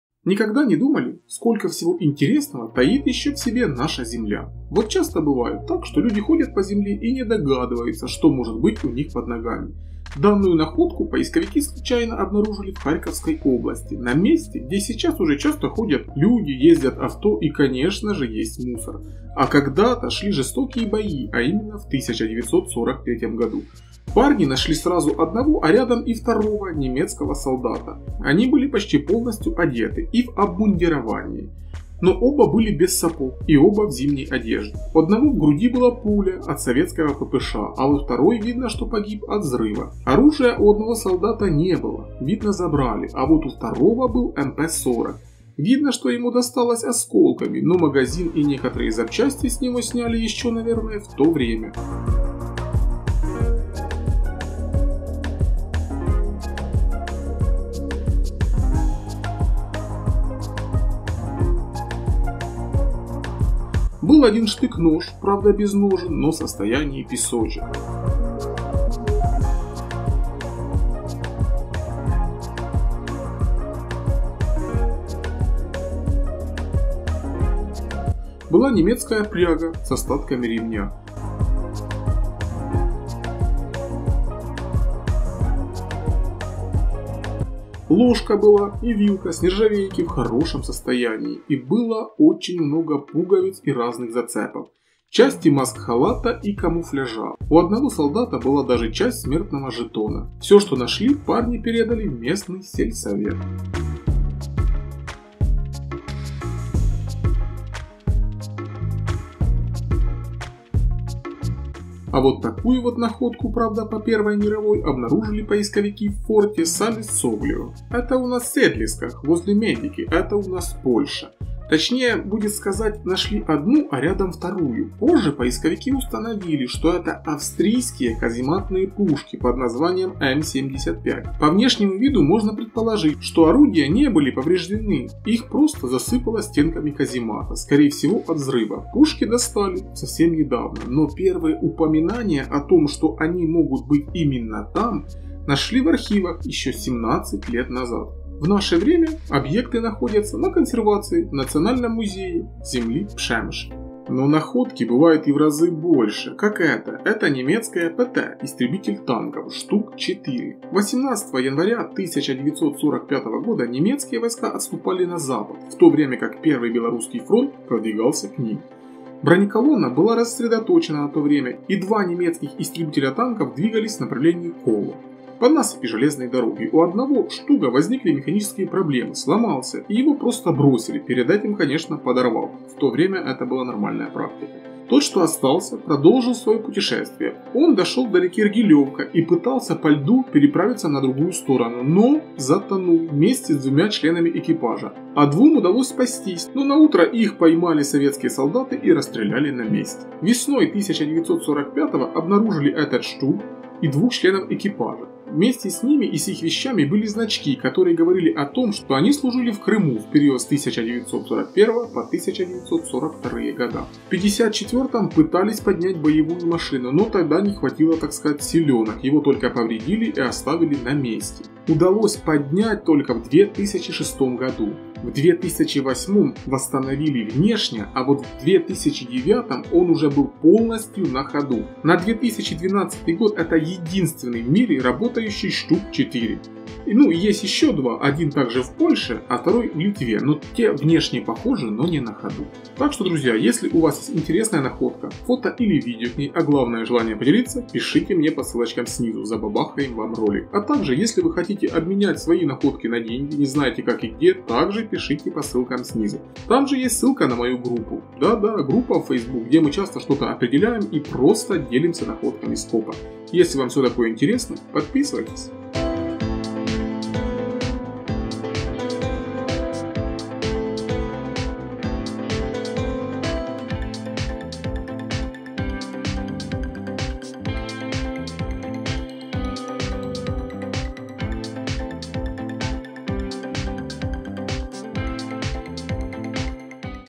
Раскопали крепость. Эти находки Вы увидите впервые.
Парни в разрушенной крепости нашли  две пушки. В реке нашли немецкий танк StuG .Находки по войне.

( On issues of COPYRIGHT and COOPERATION)